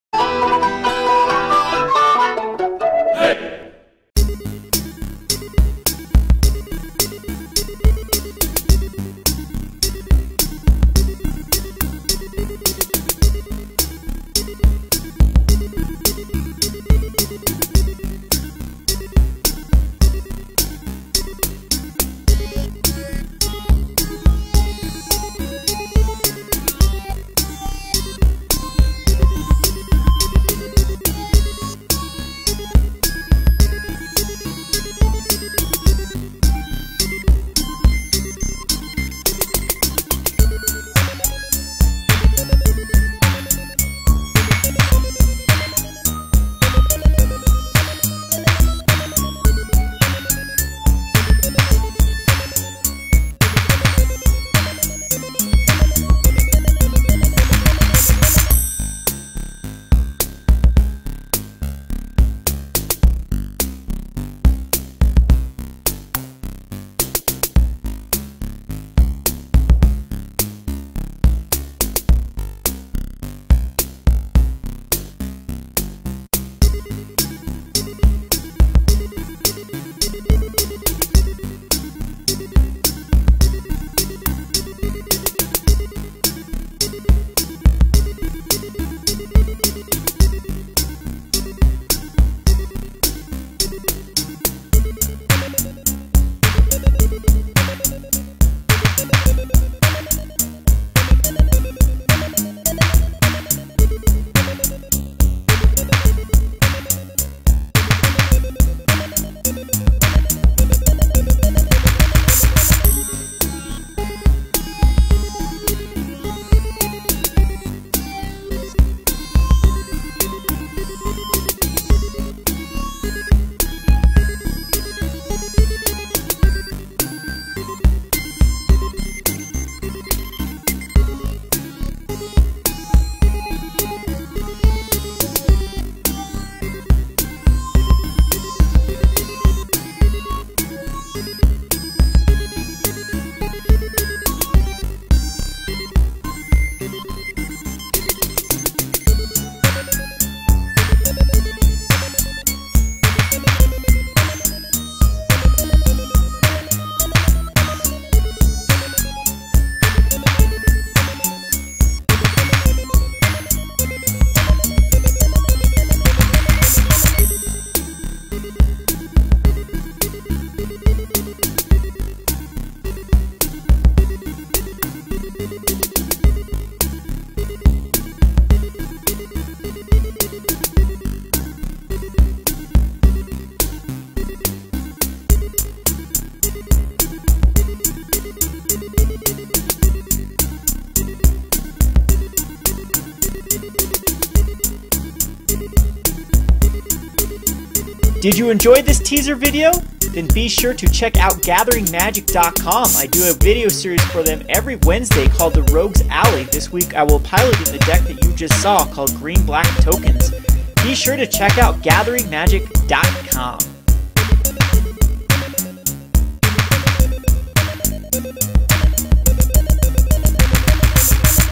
Rogue's Alley Teaser: Golgari Tokens - Magic the Gathering
Upated Decklist: 4 Cryptolith Rite
5 Forest
4 Night Market Lookout
4 Marionette Master
4 Swamp
3 Loam Dryad
4 Westvale Abbey
4 Blooming Marsh
4 Zulaport Cutthroat
4 Weaponcraft Enthusiast
2 Foul Orchard
4 Hissing Quagmire
4 Catacomb Sifter
3 Nissa, Voice of Zendikar
3 Voldaren Pariah
4 Smuggler's Copter

4 Grasp of Darkness
4 Transgress the Mind
4 Lost Legacy
3 Appetite for the Unnatural

Send us crap: Gone Rogue Games

Happy Gaming!